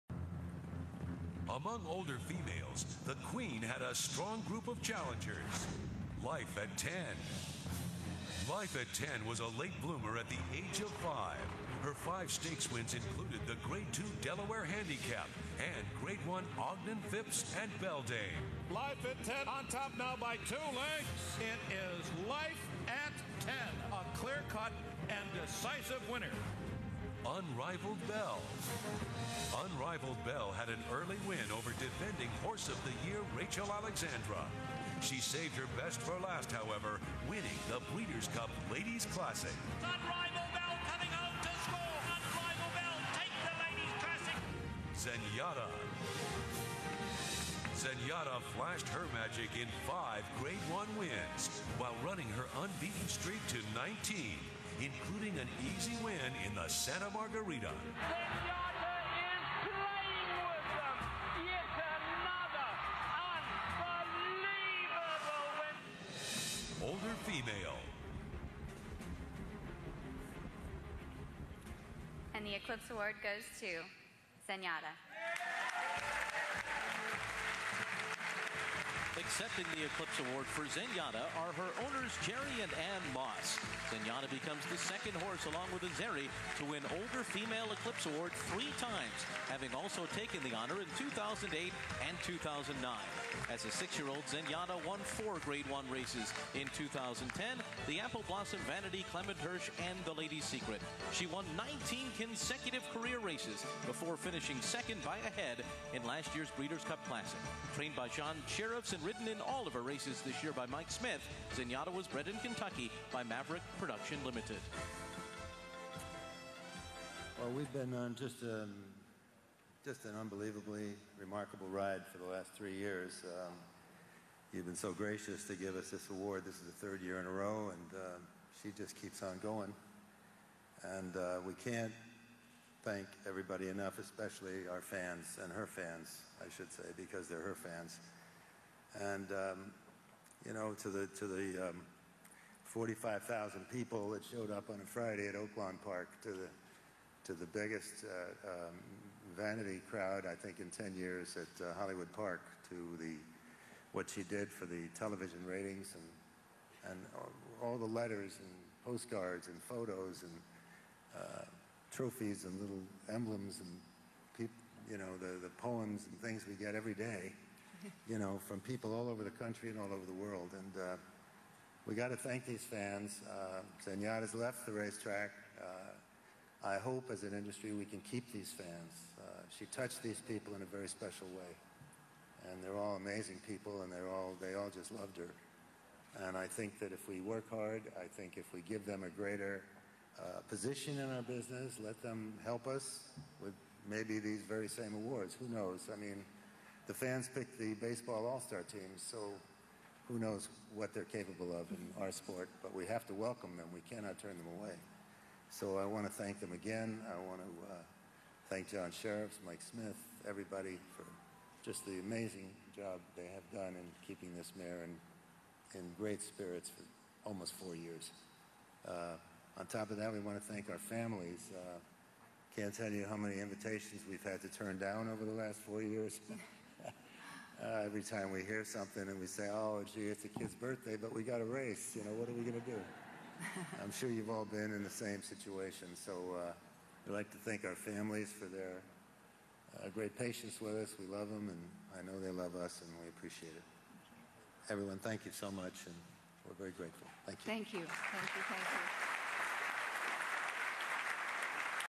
40th Eclipse Awards: Older Female
Zenyatta takes the Eclipse Award for older female on January 17, 2011.